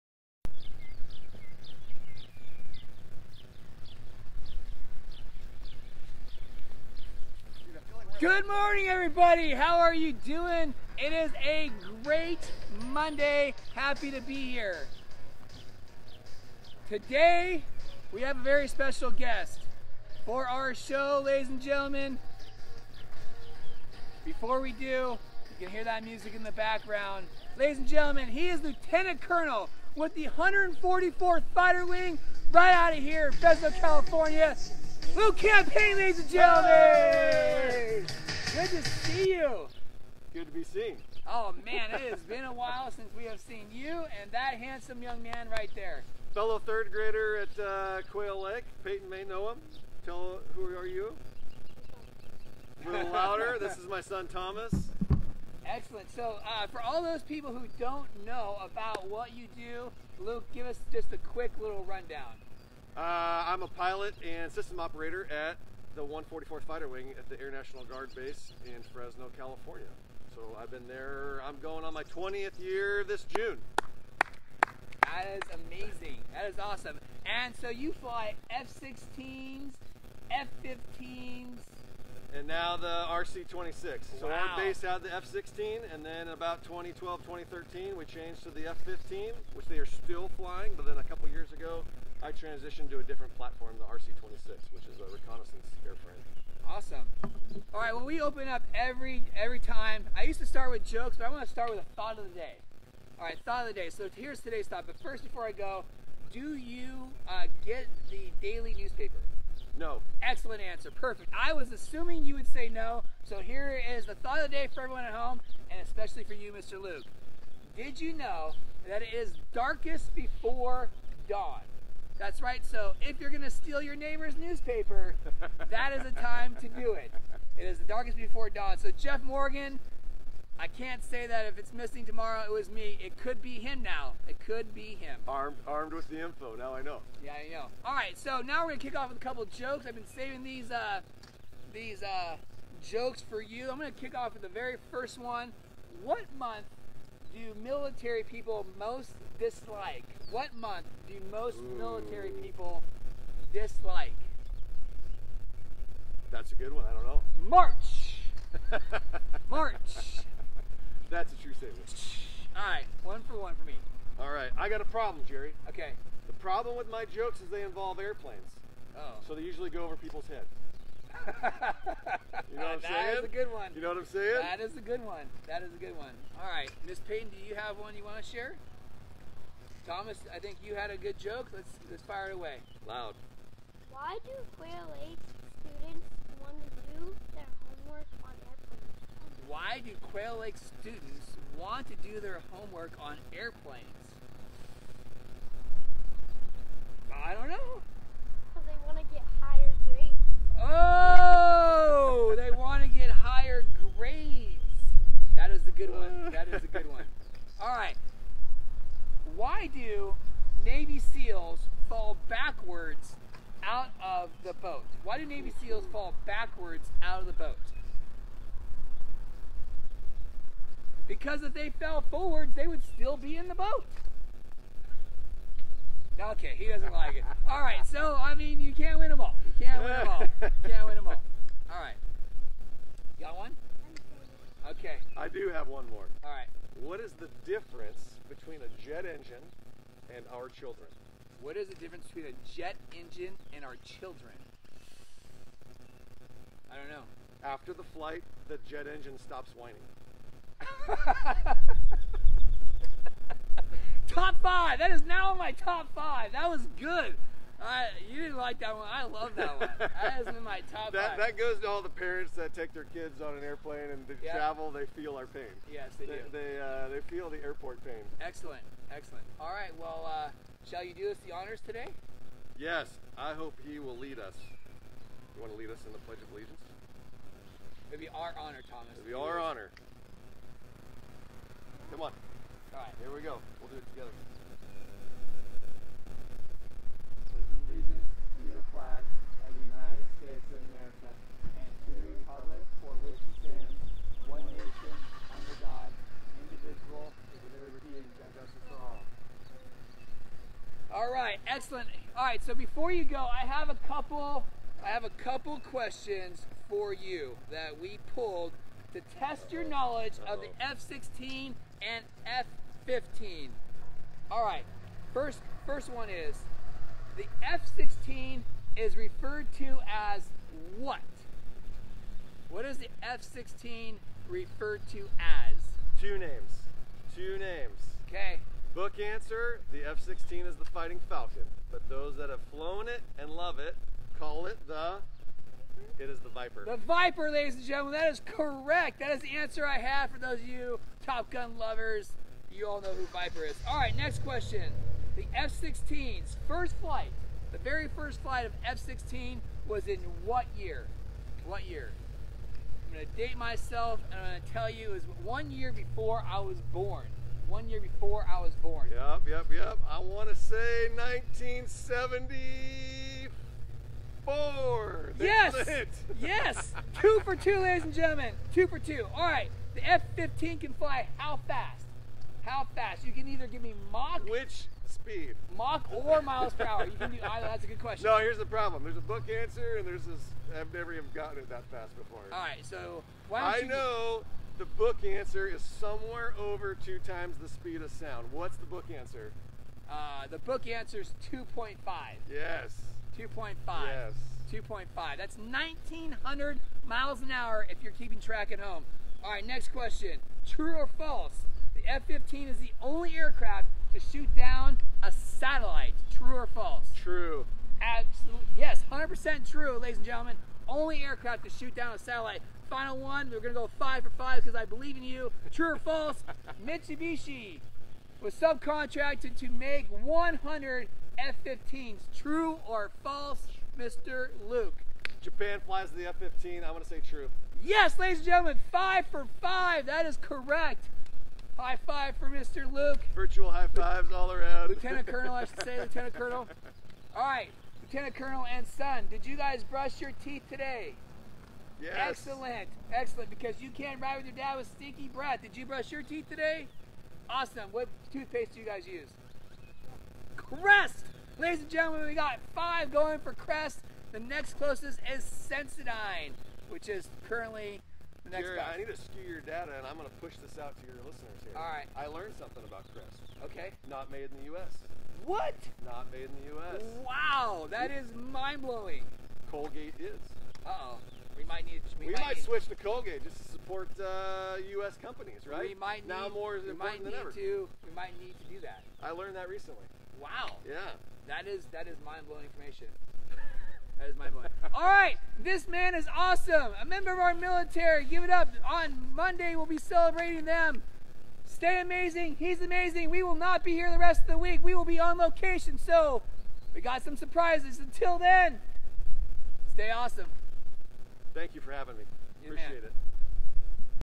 5.18.20: LT Col Luke Champagne
144th Fighter Wing, Luke joins us to share some facts, tactics and jokes.